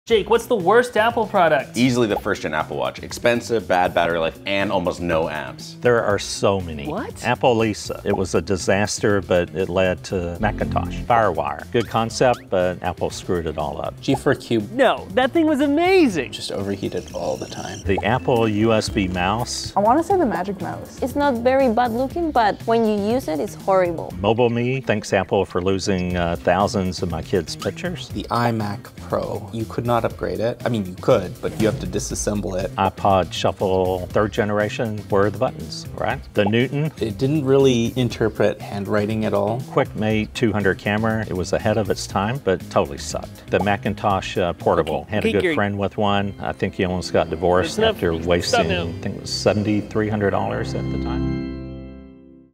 These are the WORST Apple products!
Apple's usually known for making high quality products... but what's the worst one they've made?